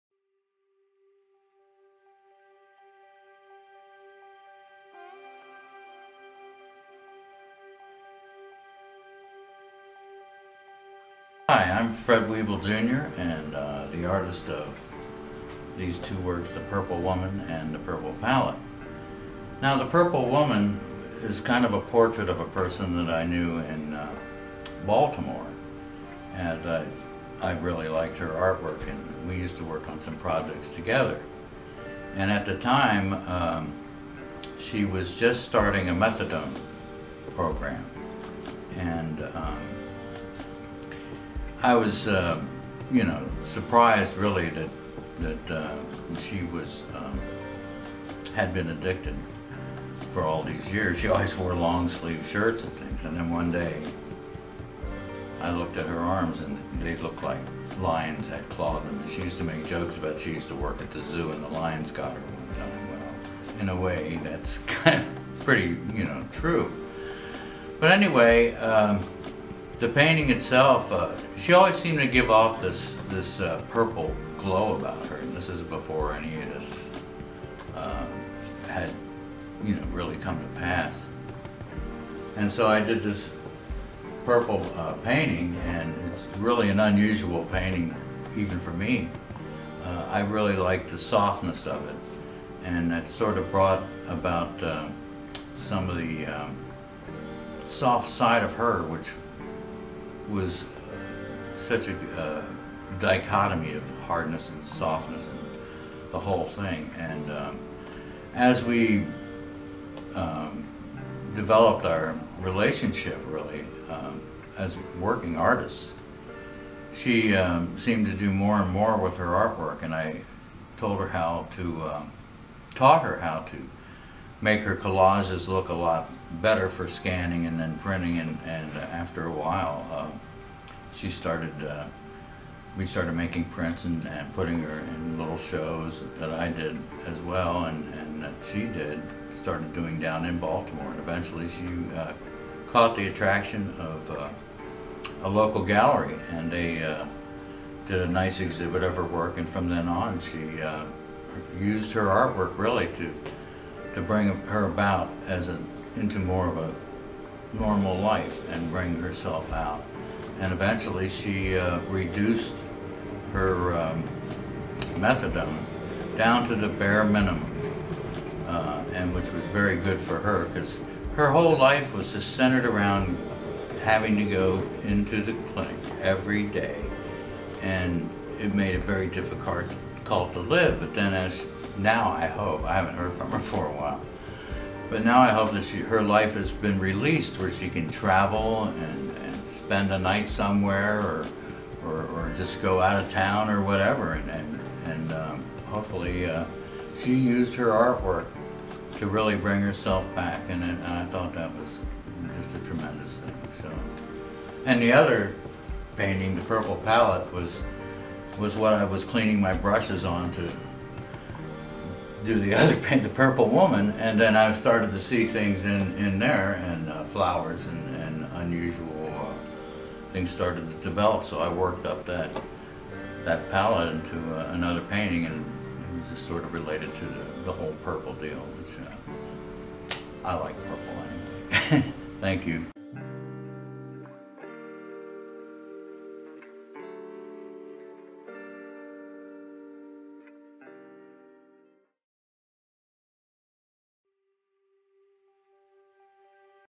Symbolizing the Hope in Recovery through Purple Artist Focus: Fred Wiebel, Jr.
Fred Wiebel, Jr. talks about his works of art in the Washington Goes Purple exhibit at the Washington County Arts Council.